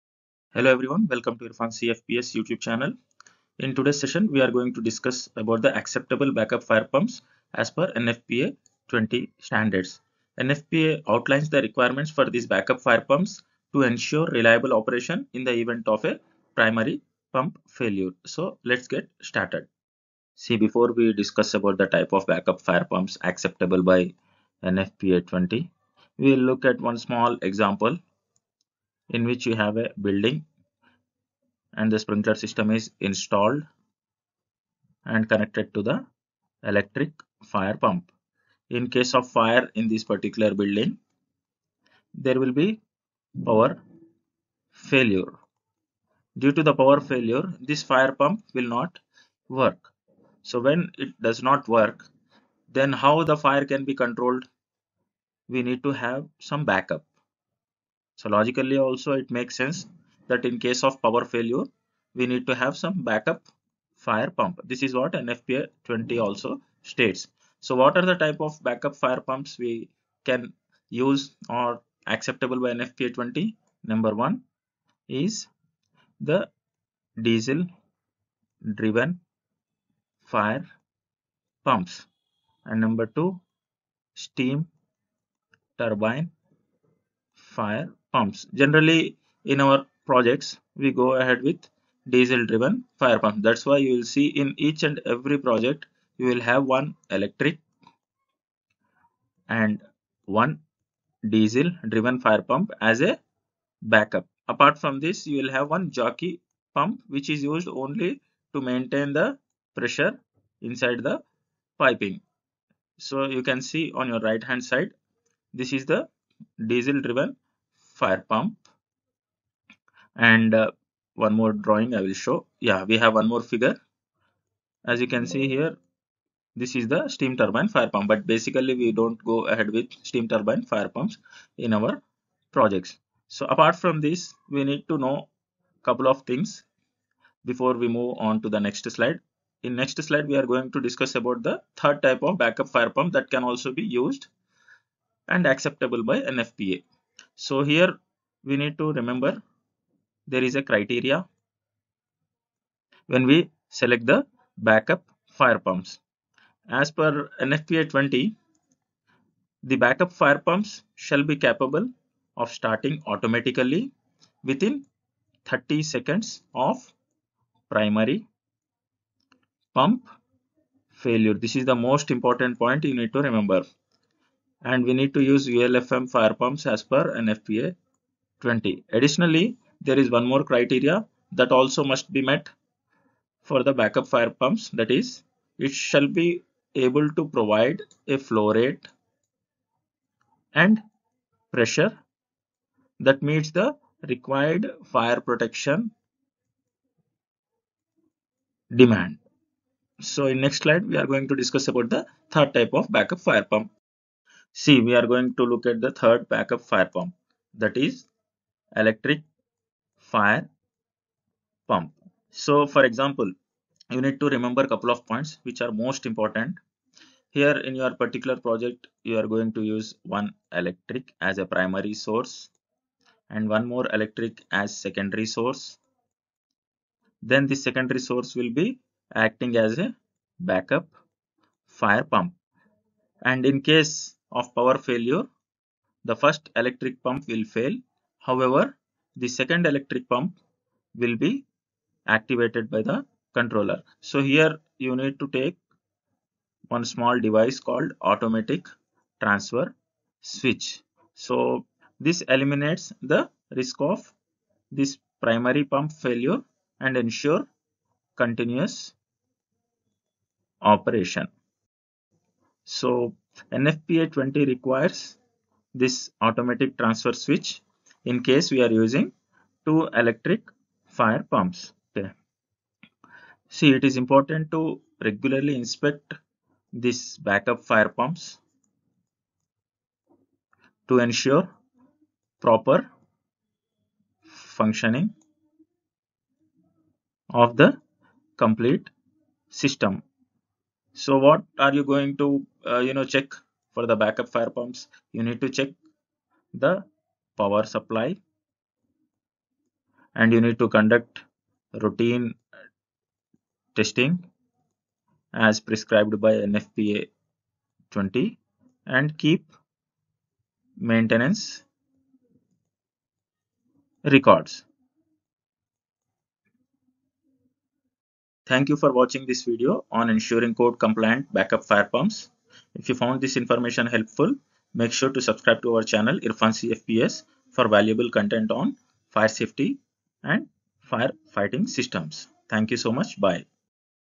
Choosing Backup Fire Pumps: Everything You Need To Know
In this video, we'll be discussing the acceptable backup fire pumps NFPA 20. This video is especially important for home and business owners who may need to use an acceptable fire pump in the event of a fire.

This video is a must-see if you live in a municipality that requires an acceptable fire pump, or if you own a business and want to be prepared for a fire. We'll discuss the different types of acceptable fire pumps, what they are used for, and how to choose the right one for your needs. After watching this video, you'll be able to make an informed decision when choosing an acceptable fire pump!
Acceptable Back up Fire Pumps NFPA 20.